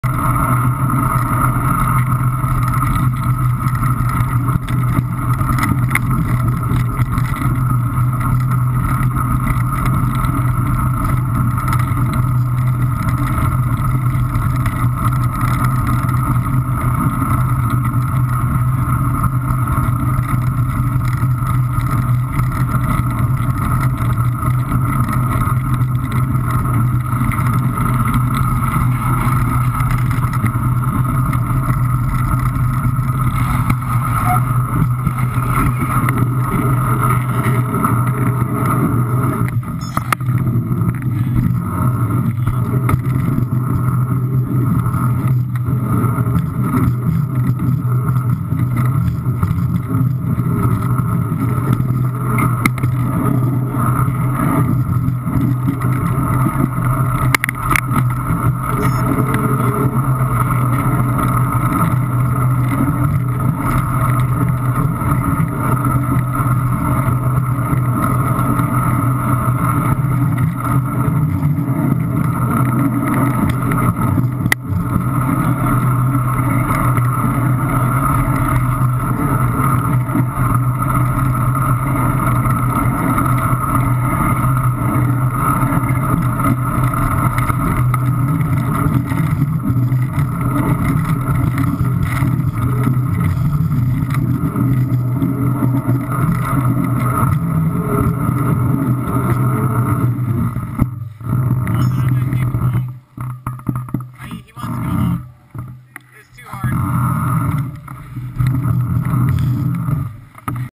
Matty rides snow
Getting the Deschutes Cup CX race was not easy when the snow started falling!